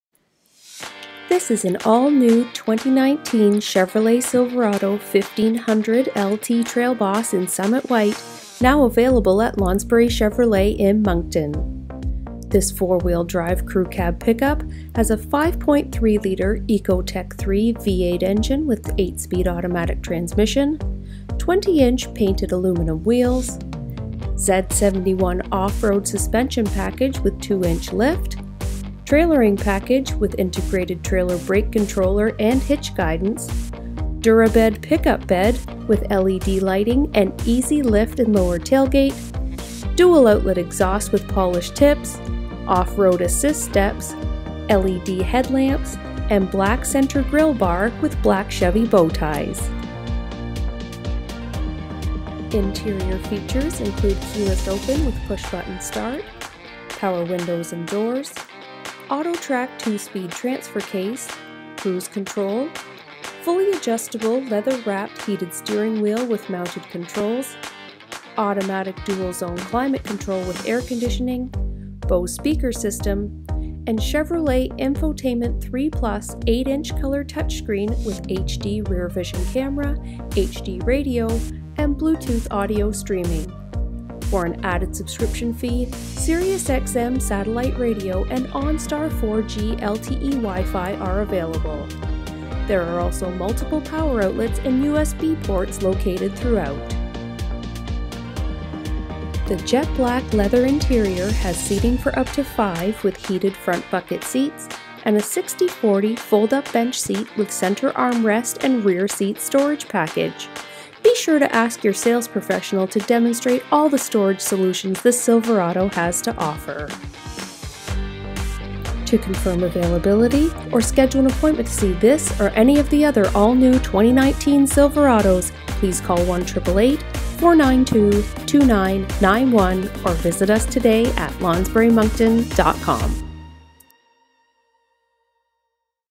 2019 Chevrolet Silverado LT Trail Boss 4WD Crew Cab | Lounsbury Moncton
All-New 2019 Chevy Silverado 1500 LT Trail Boss 4WD Crew Cab now available at Lounsbury Chevrolet in Moncton, NB (26385)

To view the listing for this vehicle on our website, please